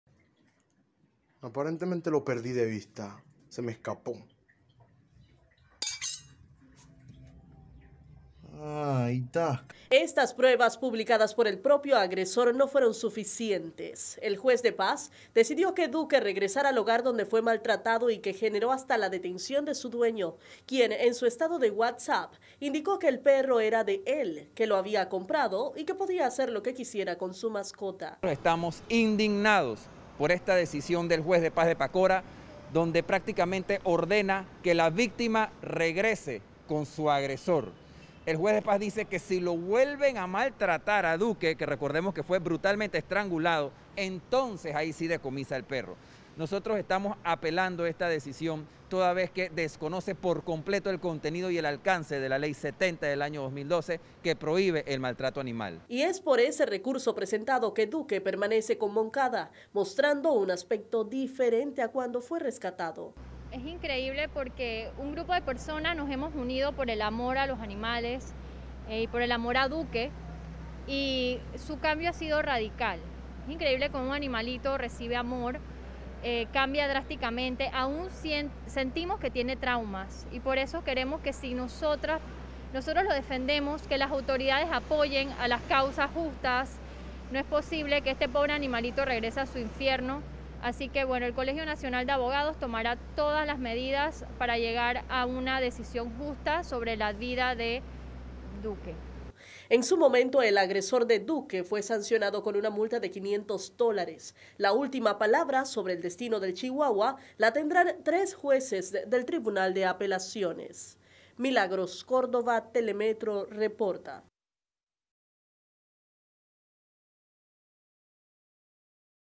Apelan fallo de devolver a perro "Duke" a casa donde fue maltratado
El juez de Paz de Pacora, ordenó que el perro "Duke", regrese a la casa con su dueño, donde fue maltratado. El Colegio Nacional de Abogados (CNA), apeló dicha decisión.